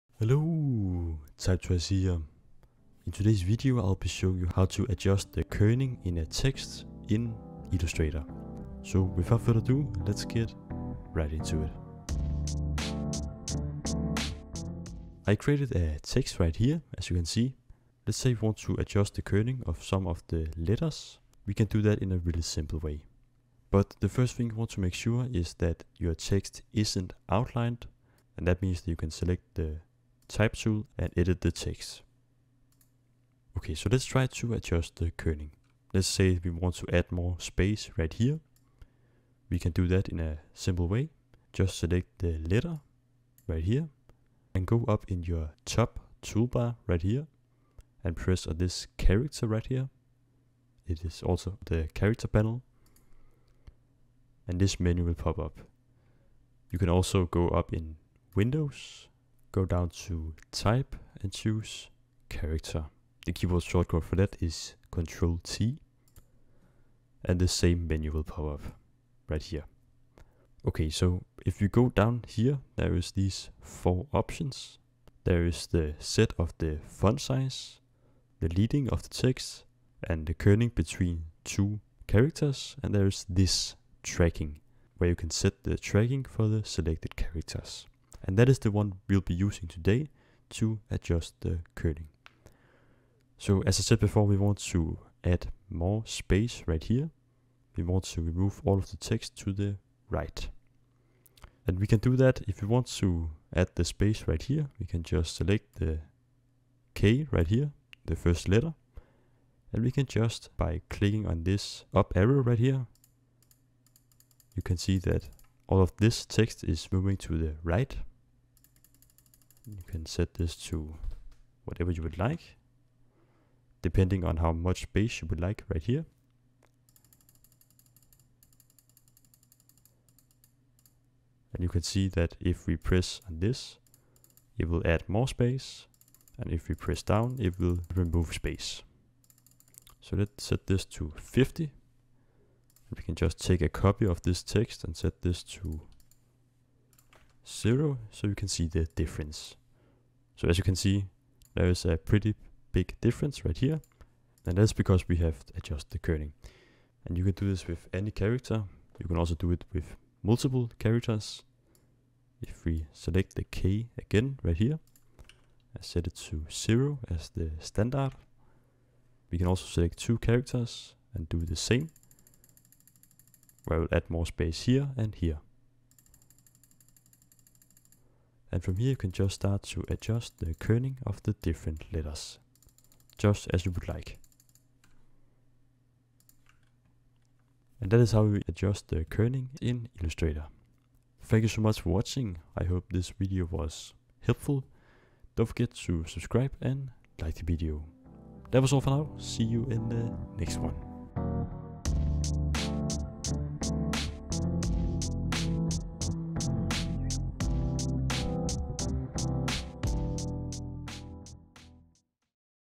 How to Adjust Kerning in Adobe Illustrator
In today's video, I'll be showing you how to adjust kerning and letter spacing for text in Adobe Illustrator CC. Enjoy the video!